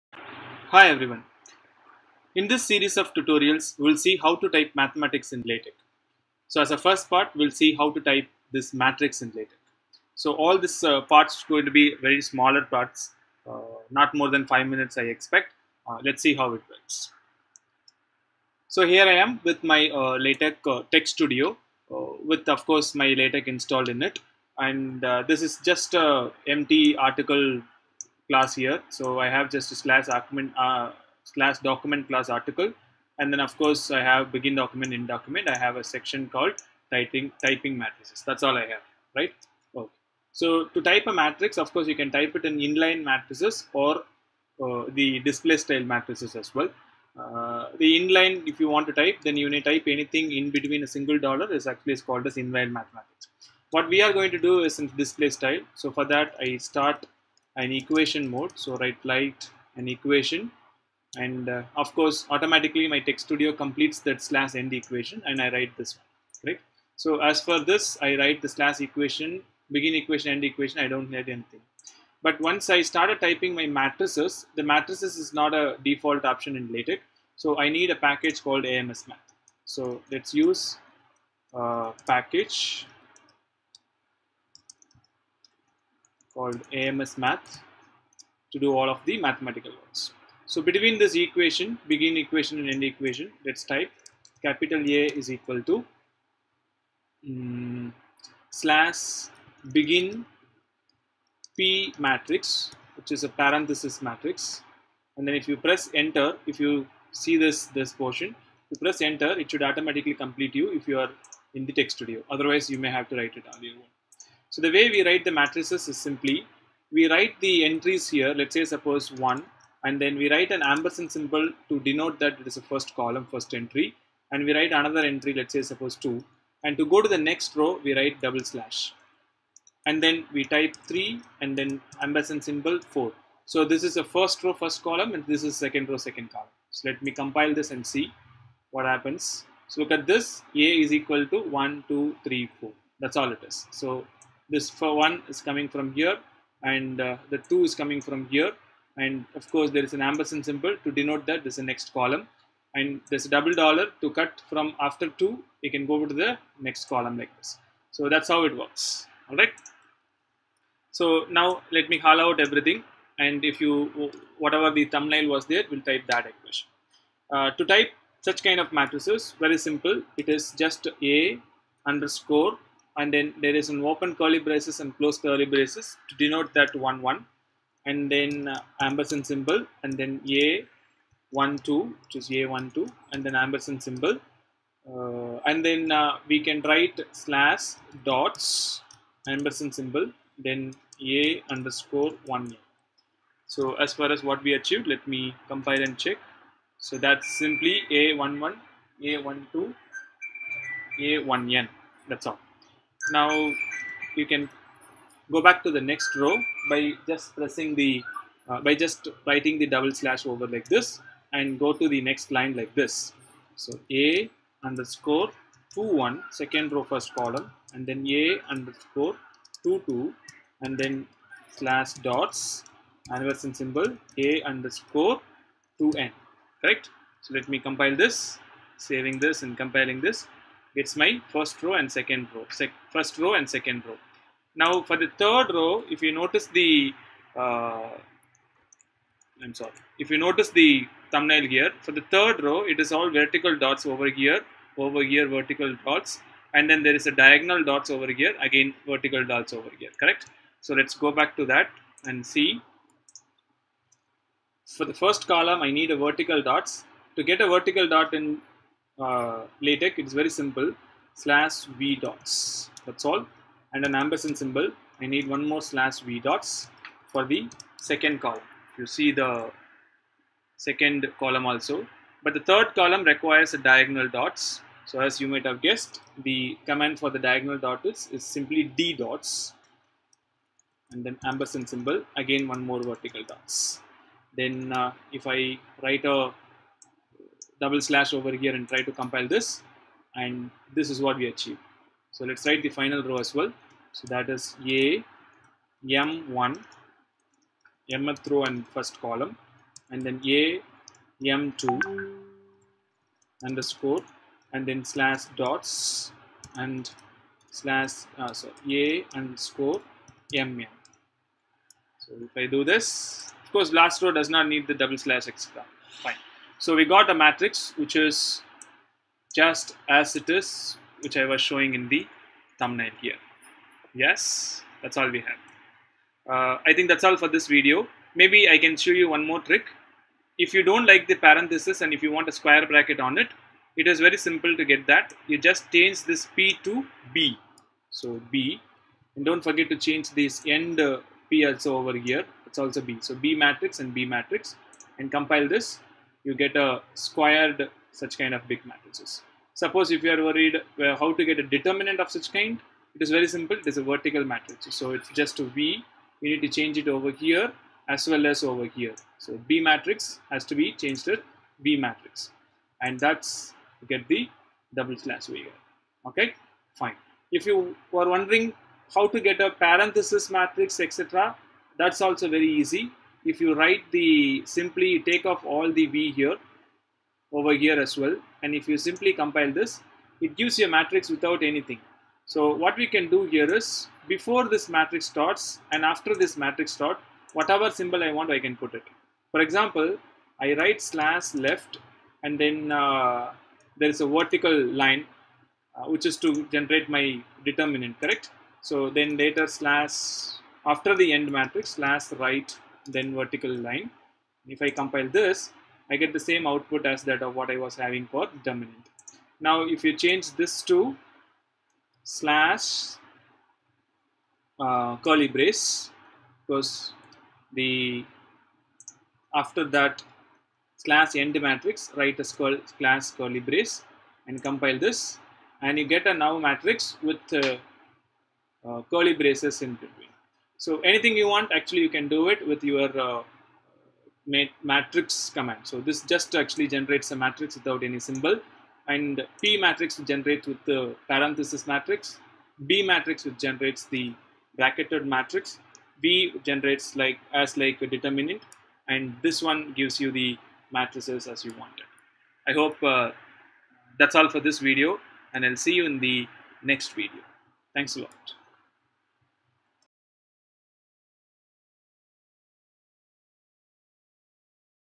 Part 01: Typing Matirces in LaTeX
Hai Everyone,

In this video, we will learn how to type matrices in LaTeX.